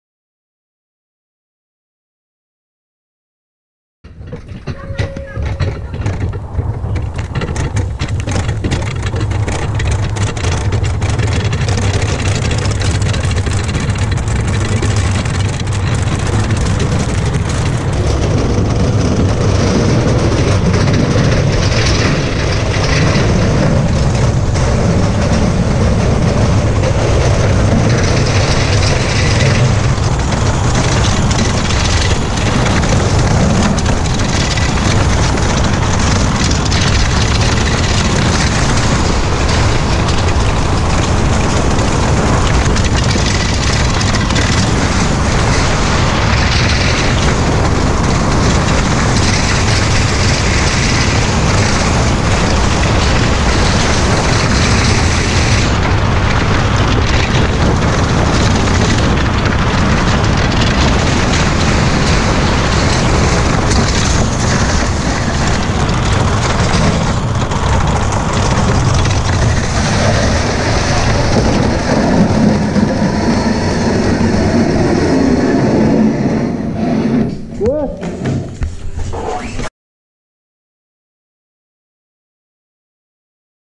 Tristan Schwandke Bobsleigh with Beat Hefti - Kunsteisbahn Bob-Rodel Igls
Tristan Schwandke - Bobsleigh with Beat Hefti - Innsbruck-Igls